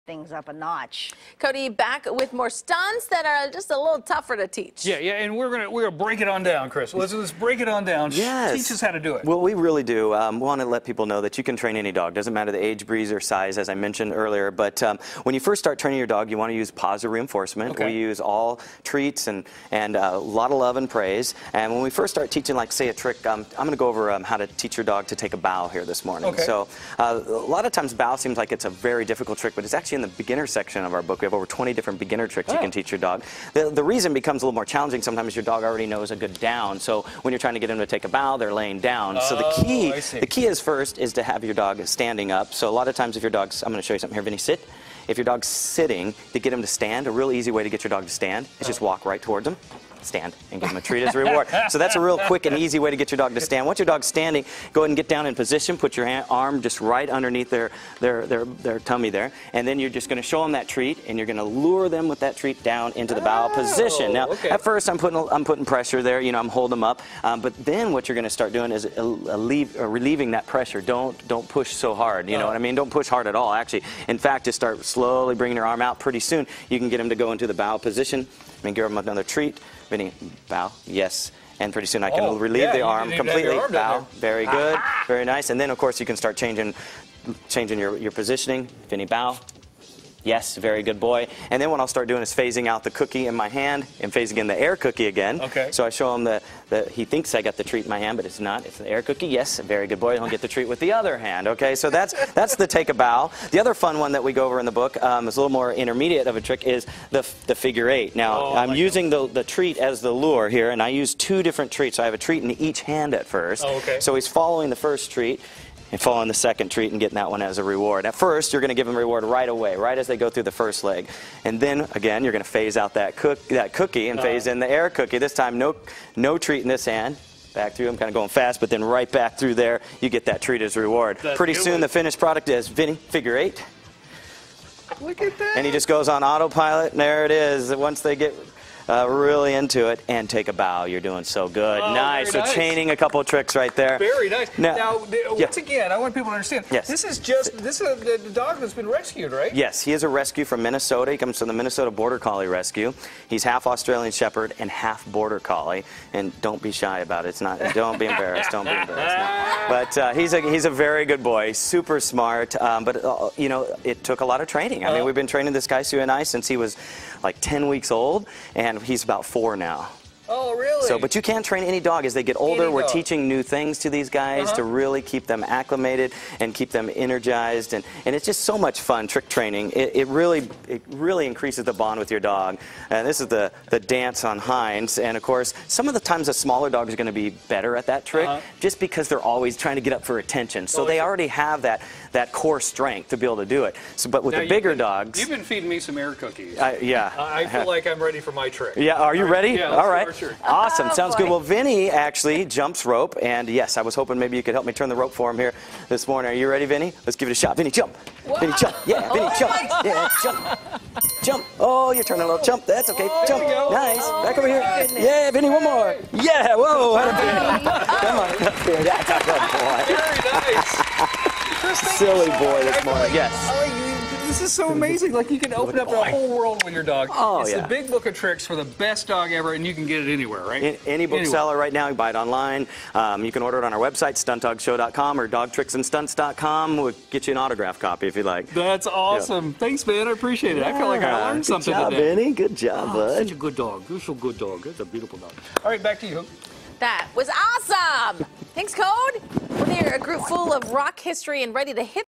Big Book of Dog Tricks Pt 2
Chris Perondi and his pups are showing off some of the tricks you can teach your dog.